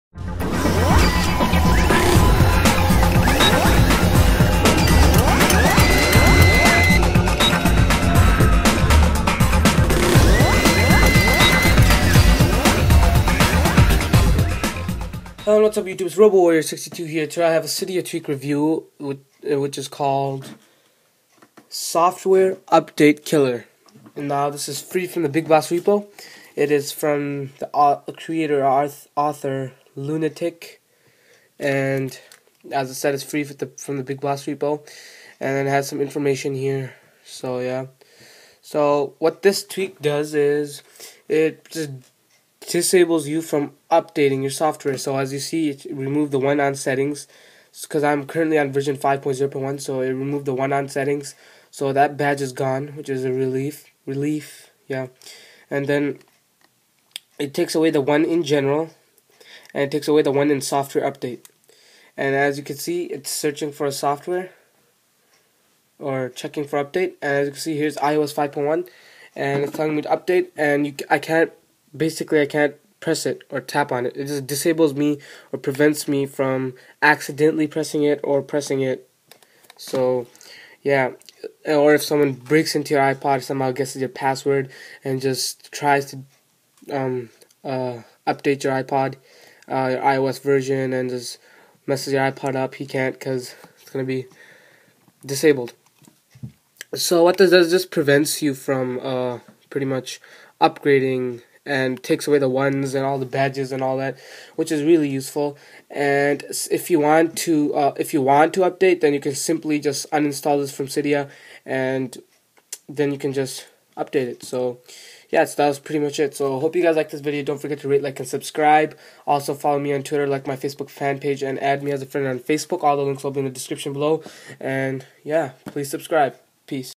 Software update killer
Hi guys and today i am going to be showing a ver cool cydia tweak which disables or prevents you from accidentally updating your iOS version. If you want to update then just simply uninstall this tweak.